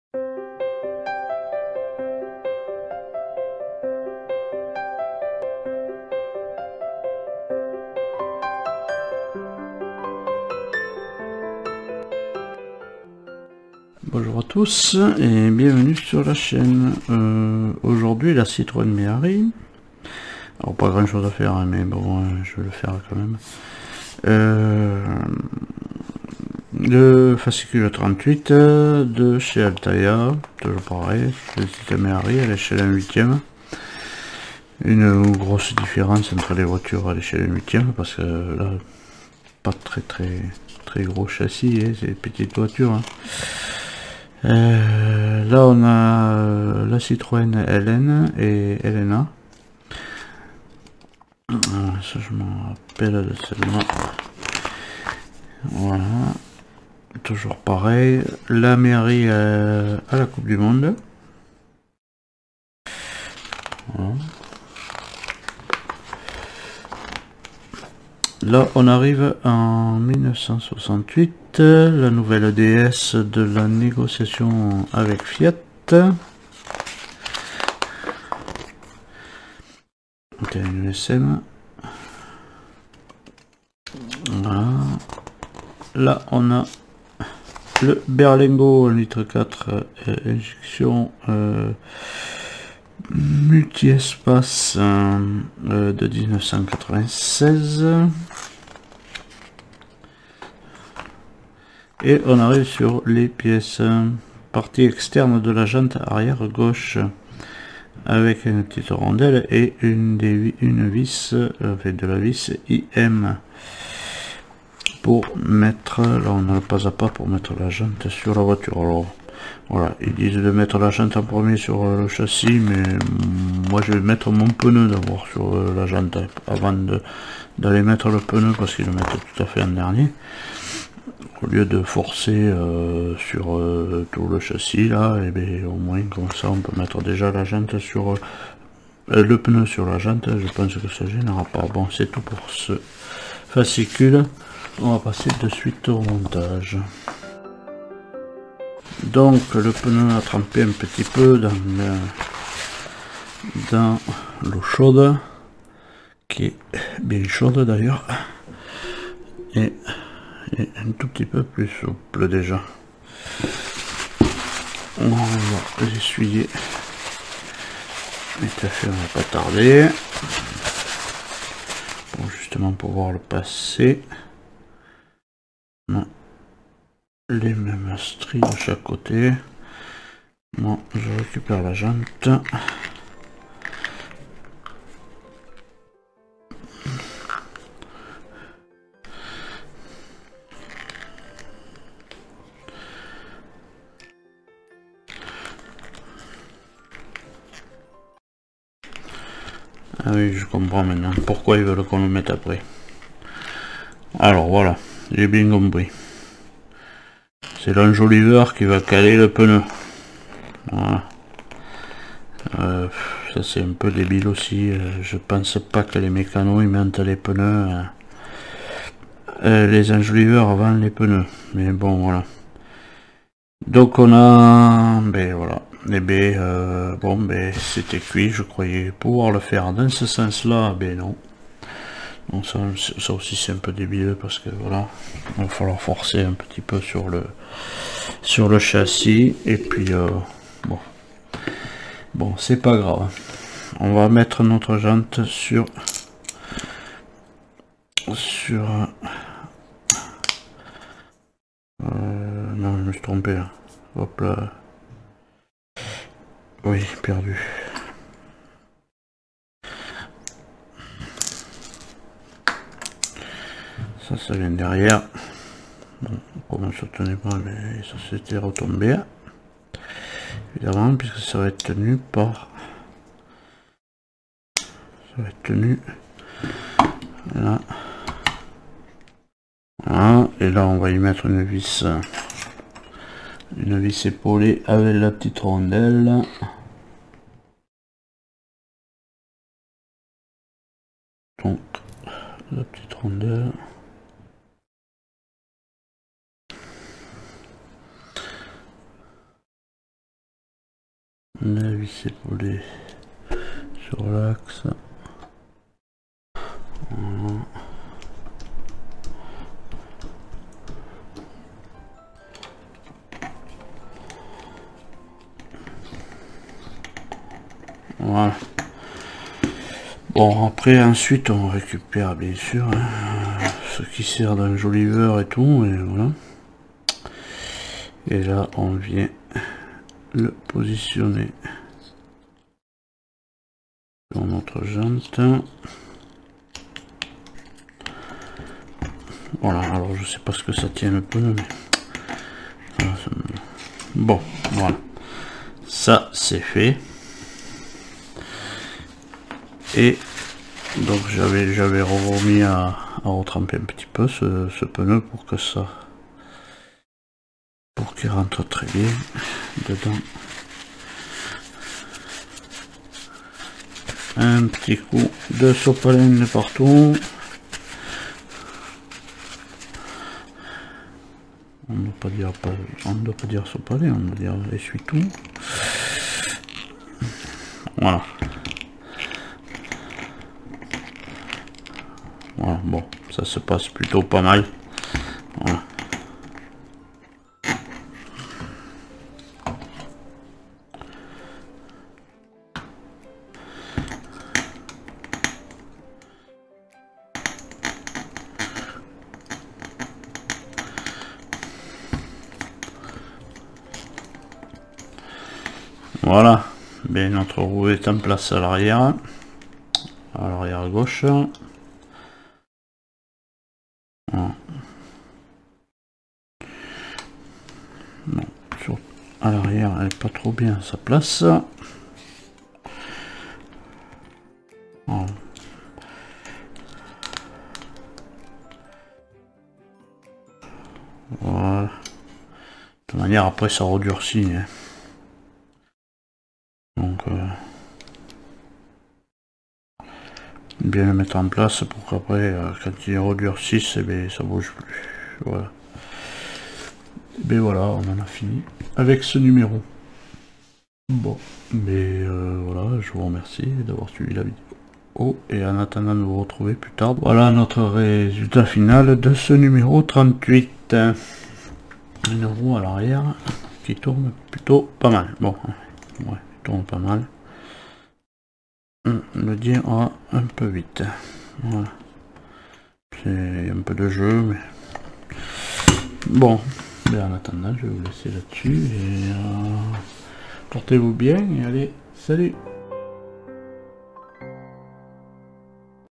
la citroën mehari de chez altaya n° 38
Une voiture française la mehari transport de tous les loisirs. Montage de la jante et du pneu sur le châssis.